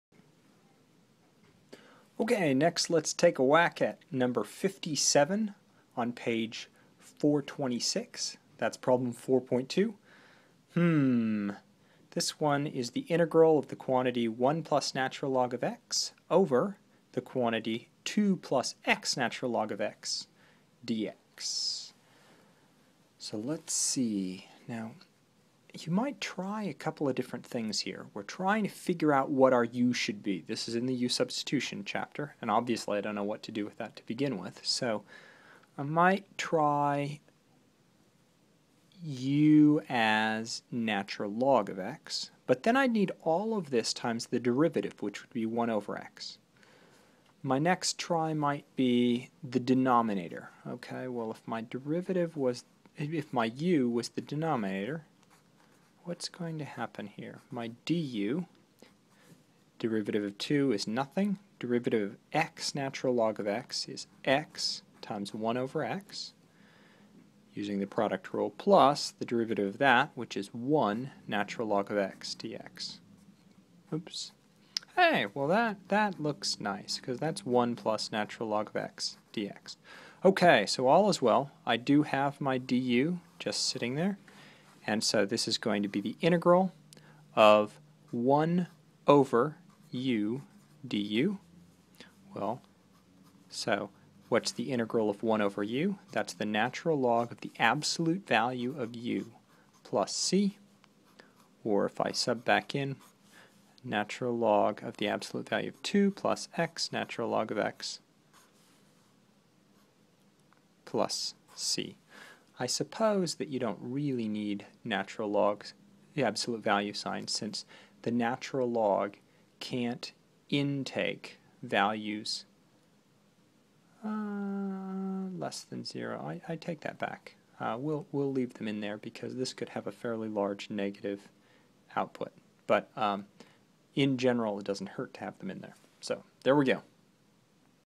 U-Substitution with an Indefinite Integral including Fractions and Logarithms
We calculate an indefinite integral involving factions and logarithms by using u-substitution.